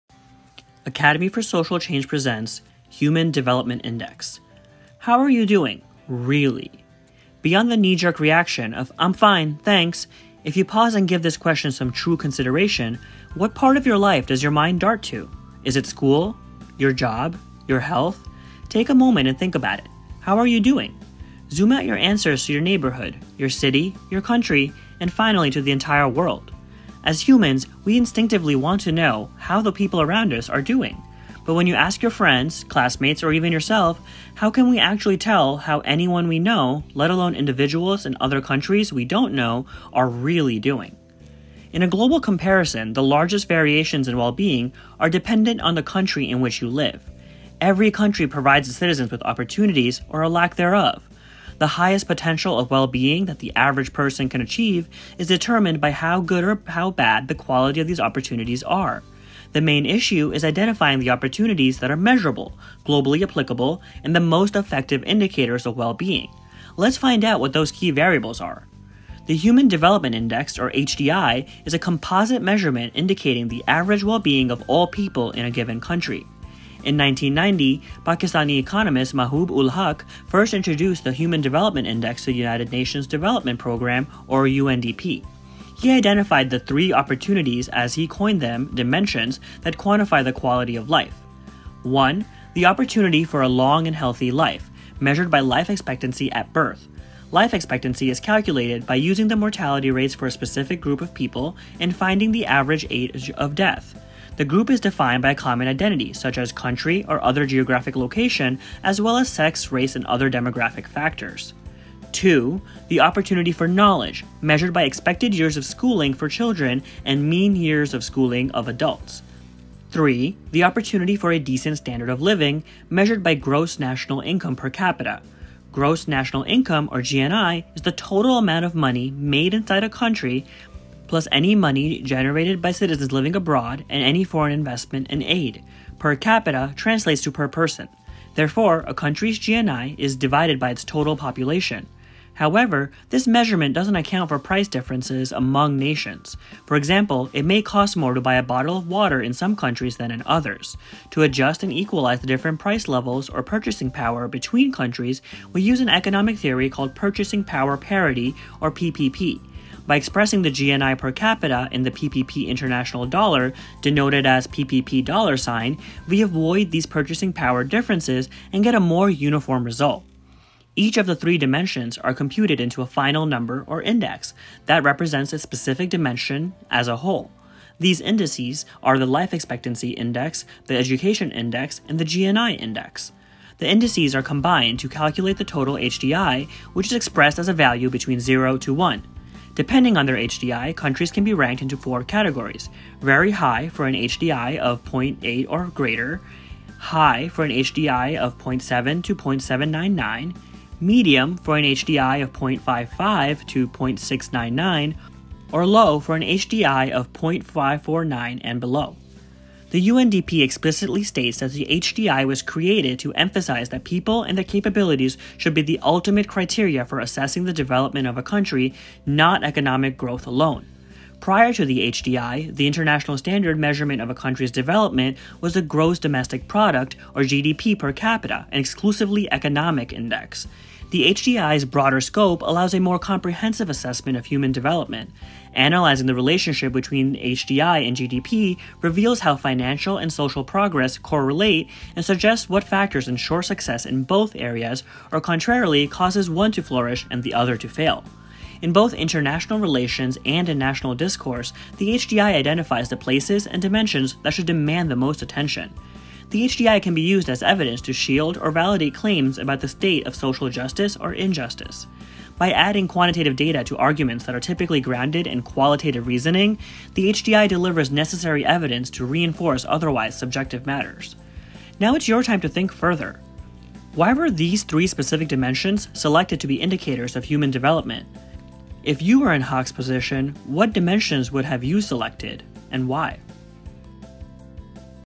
Human Development Index: Quantifying Quality of Life - Development Economics Series | Academy 4 ...
The Human Development Index, or HDI, is a composite measurement indicating the average wellbeing of all people in a given country.

**Think Further Questions:
1. Why were these three specific dimensions selected to be indicators of human development? If you were in Haq’s position, what dimensions would have you selected, and why?
2. What other potential conclusions can you draw from analyzing any given country’s HDI compared to its GDP?
3. Why do you think the international community historically has valued the GDP over HDI? In what ways is the HDI more important than the GDP, and vice versa?

**Contents
00:00 - How Do You Quantify “Quality of Life”?
00:41 - The Answer
01:08 - Definition: Human Development Index (HDI)
01:16 - How It Works
03:29 - Using It